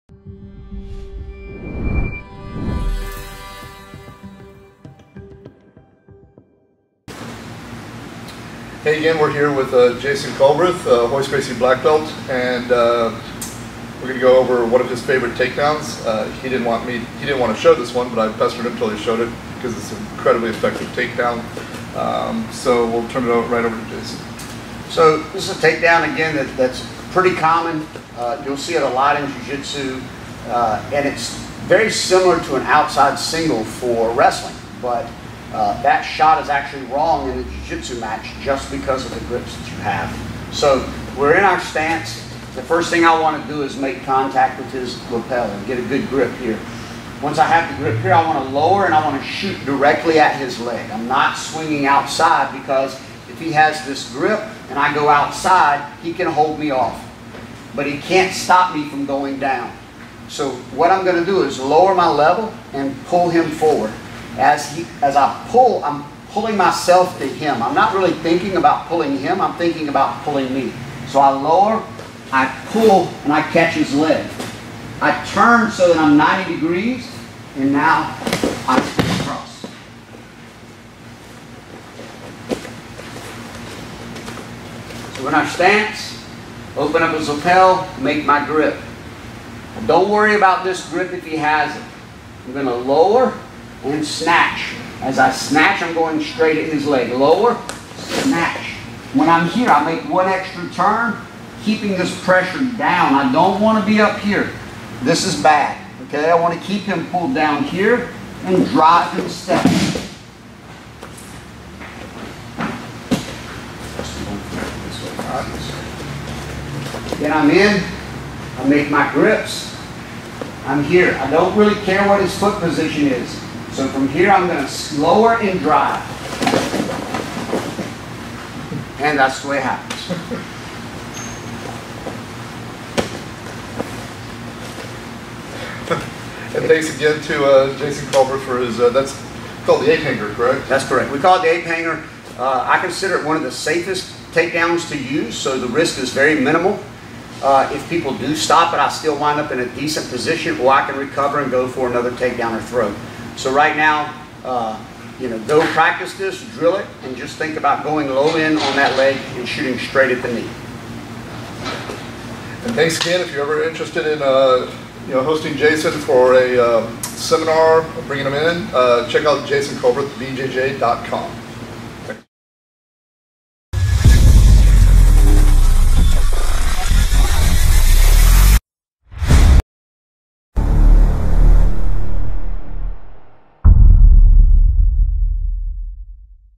A Simple but High Percentage BJJ Takedown | The "Ape Hanger"
I joined Masters World Champion, Jason Culbreth (Royce Gracie Black Belt) at his academy and he showed me one of his favorite takedowns.

This is a great takedown that doesn't take a lot of athleticism or agility and is easy to recover from if you miss it.